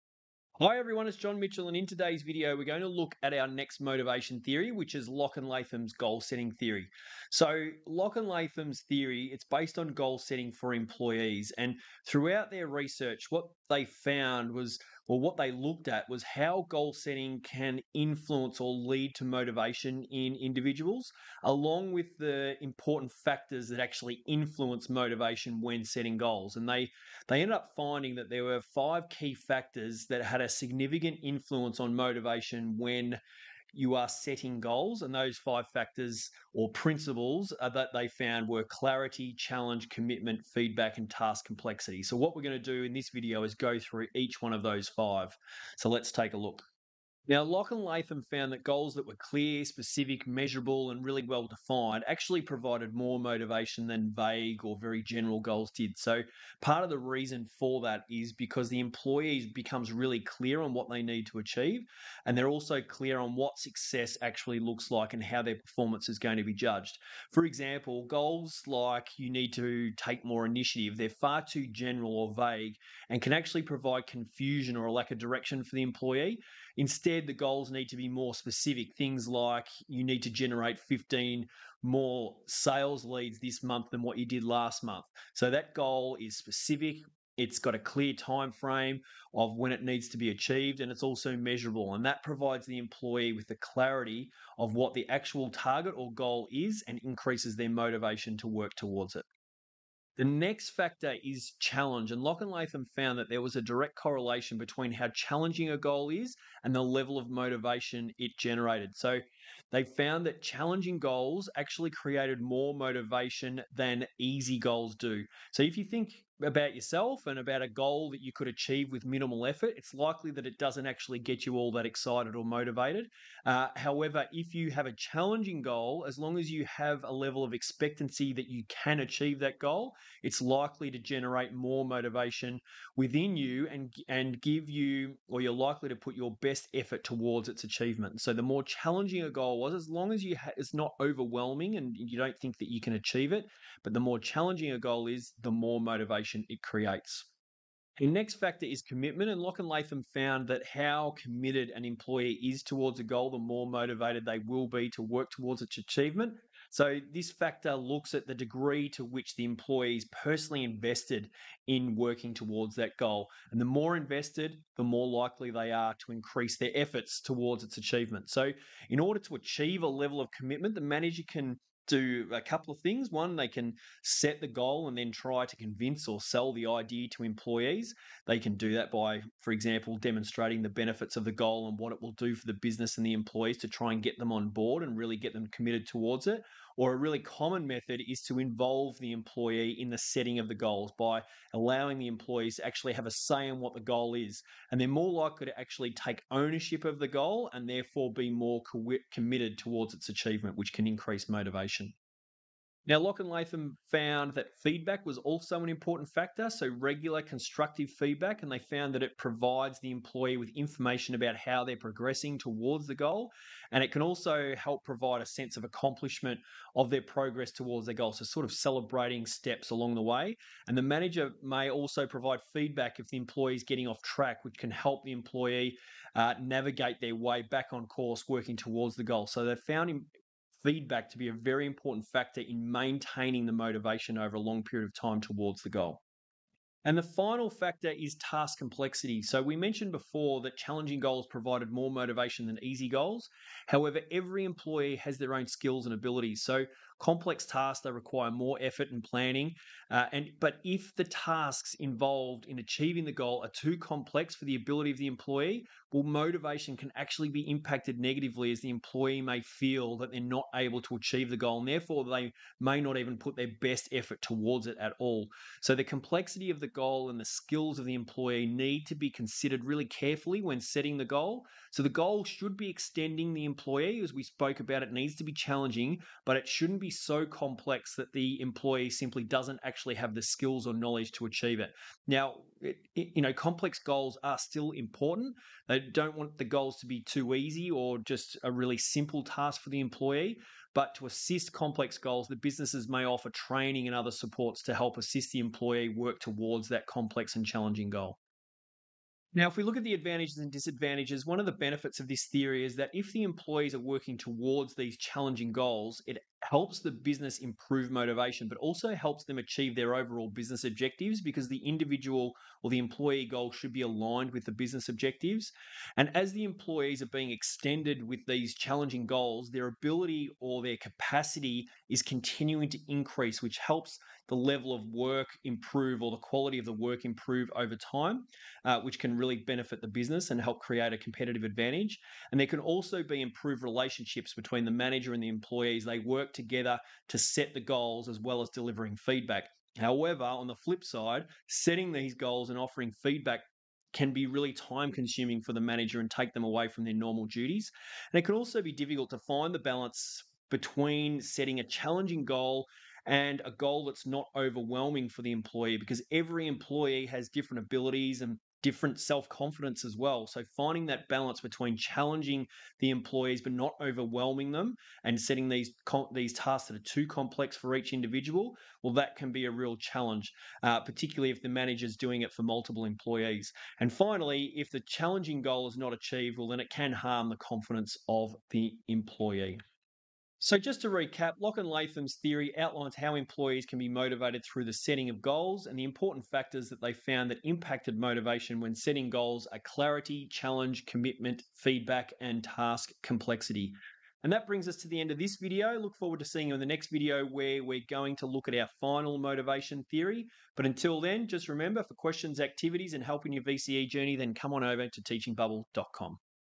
Locke & Latham's Goal Setting Theory | VCE Business Management
This video looks at Locke and Latham's goal setting theory and how it can provide motivation for employees. To access the questions for this video, come on over to https:teachingbubble.com

If you need the chapters for this video, here they are:
0:00 Intro
0:47 Clarity
1:40 Challenge
2:28 Commitment
3:22 Feedback
3:57 Task Complexity
4:59 Advantages and disadvantages
6:20 Recap